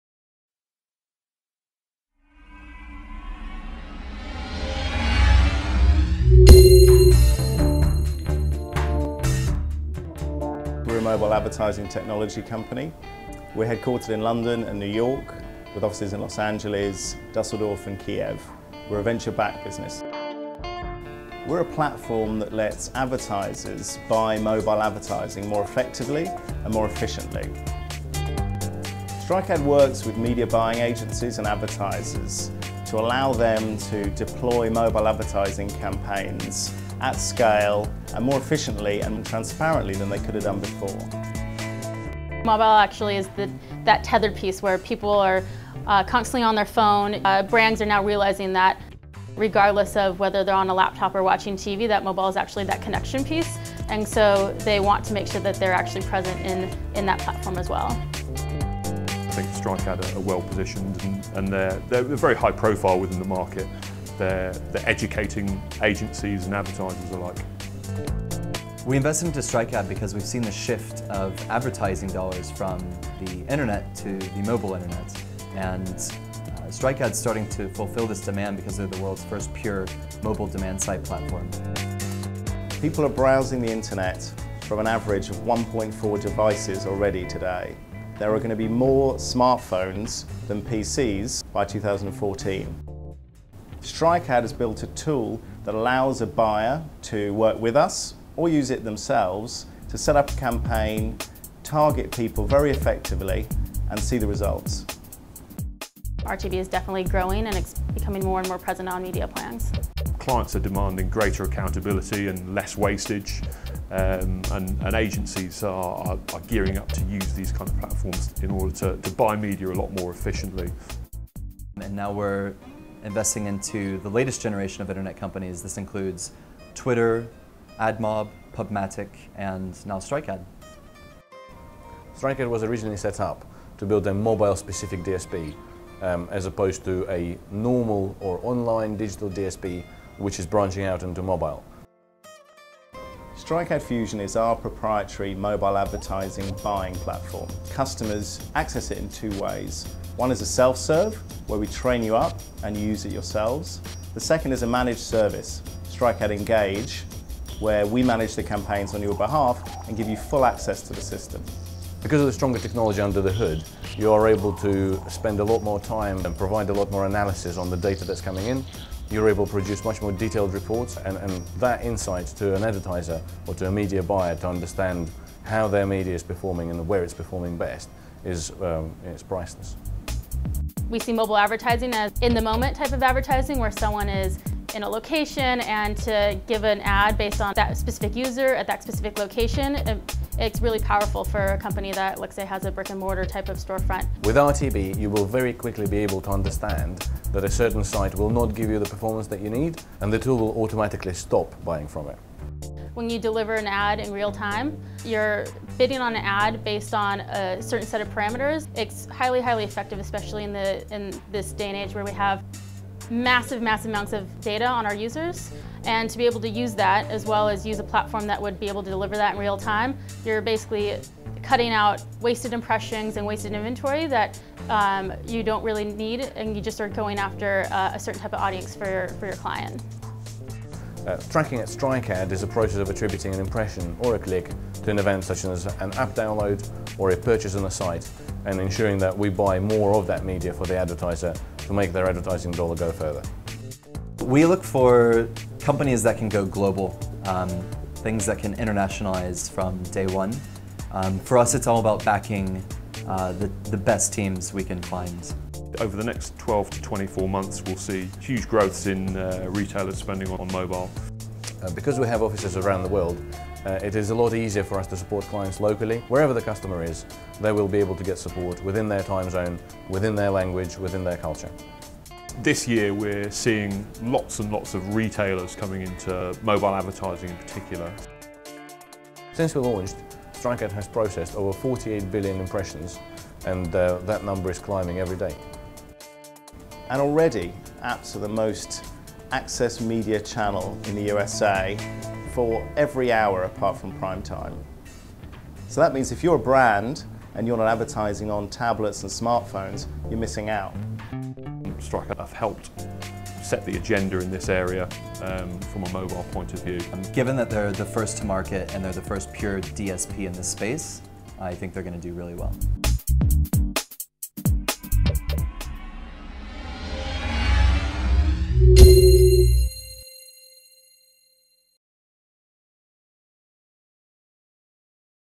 StrikeAd: The Movie (5min edit)
Whether you're a digital agency or a leading brand, StrikeAd makes it easy for you to set up, optimize and deliver powerful and effective mobile advertising campaigns, using the world's first demand side platform dedicated to mobile.
The result? Highly targeted, cost effective and efficient mobile ad campaigns that get you the results you want.